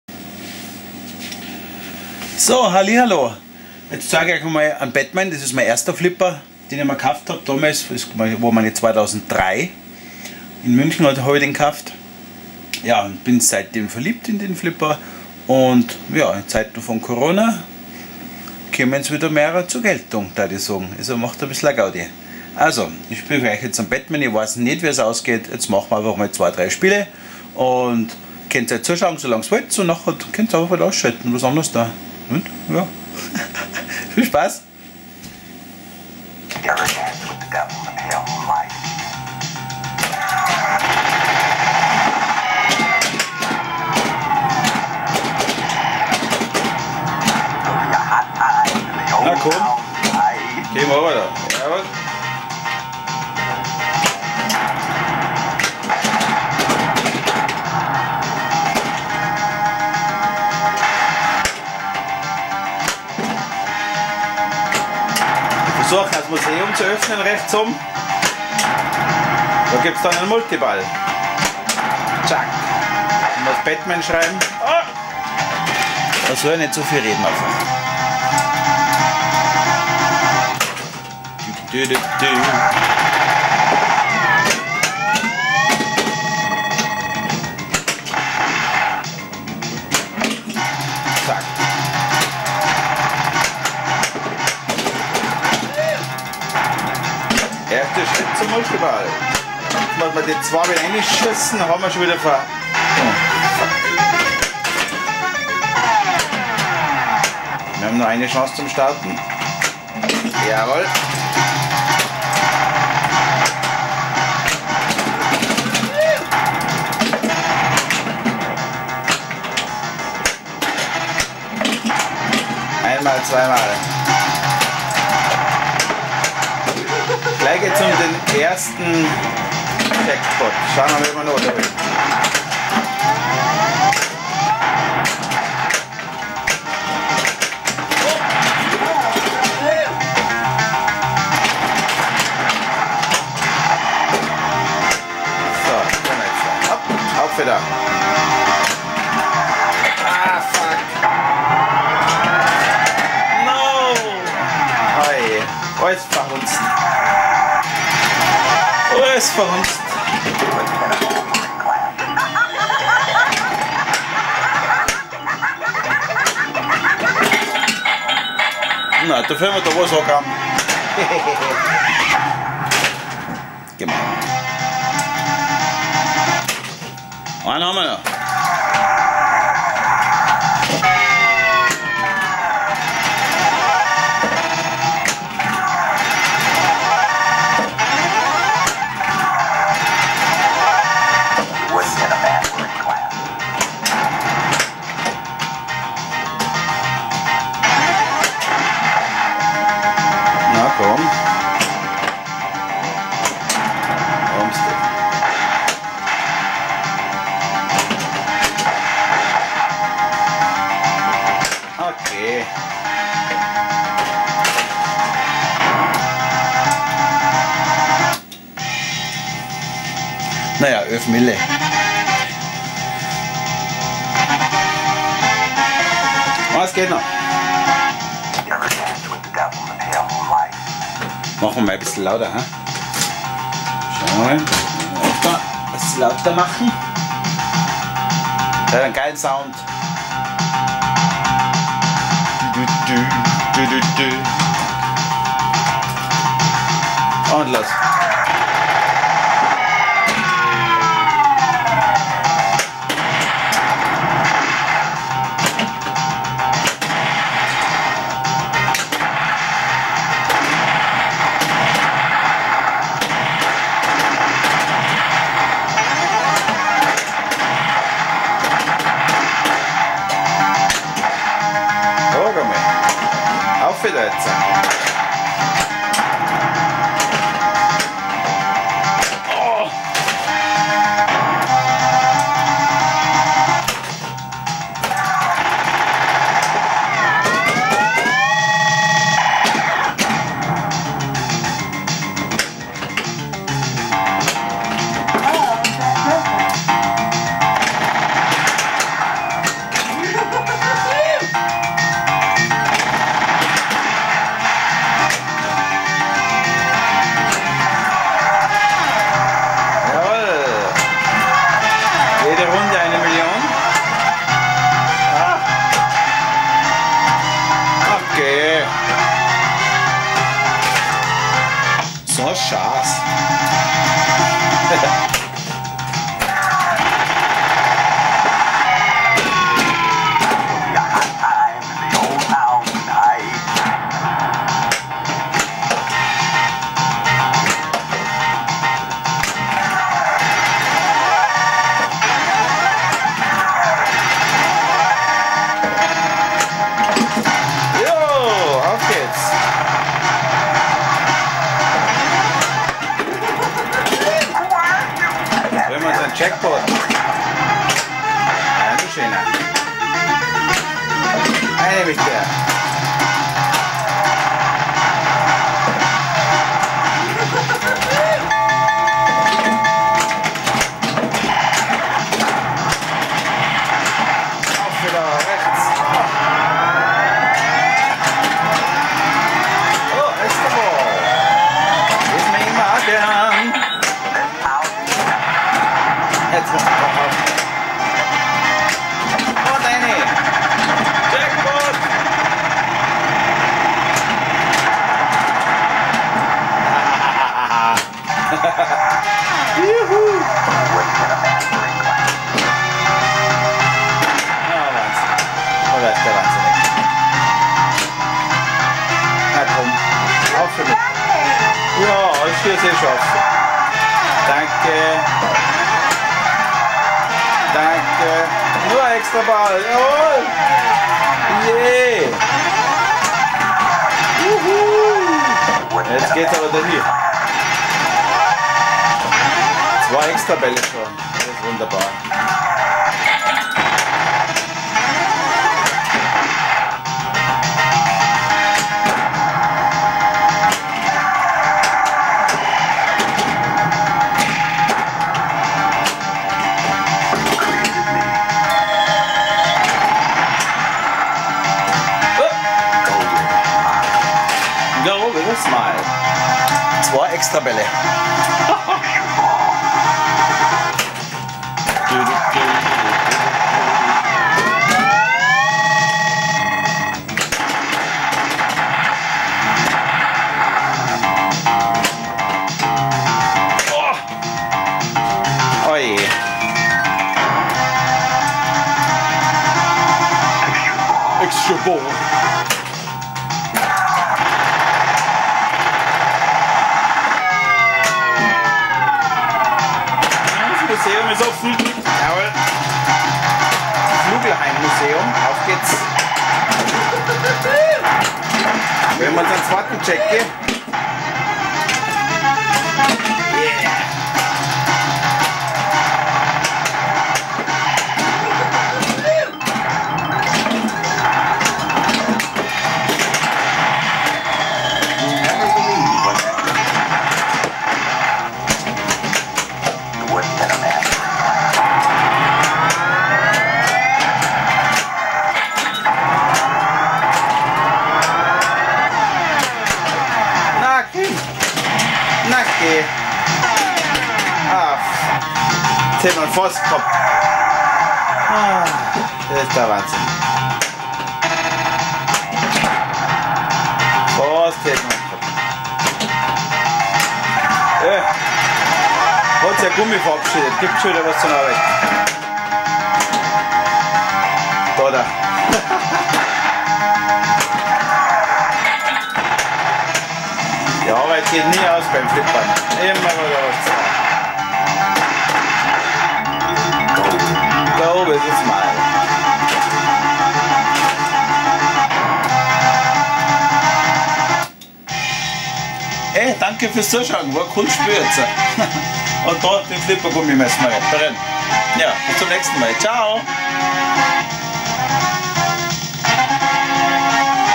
Herby´s Flippersammlung - BATMAN von Data East
Batman - Data East, mein erster Flipper. Er löste meine Sammelleidenschaft aus.